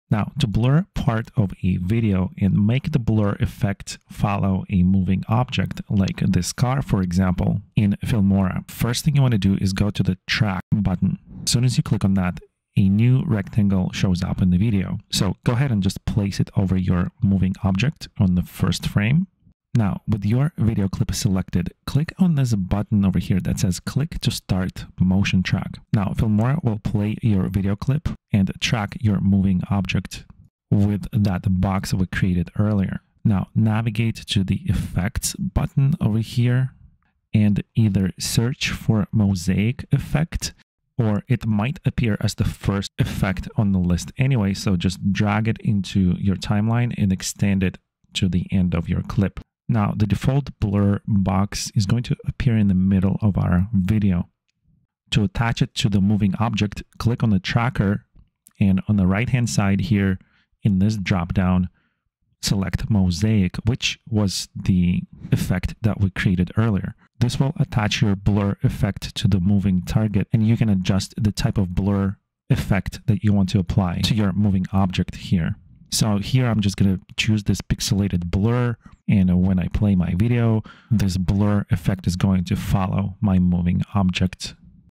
How To Blur Part Of A Video Clip And Make It Follow Moving Object or Face In Filmora
filmora videoediting how to add blur to part of a video clip, and animate it to follow a moving object, like a face, license plate, etc.

Video Chapters:
00:00 Blur Intro
00:07 Add Tracking Box Around Moving Object
00:19 Motion Track An Object
00:32 Add Blur Effect
00:54 Link Blur To Motion Tracked Object

Search phrases:
how to make blur follow face
how to blur a face in filmora
how to add blur to a moving object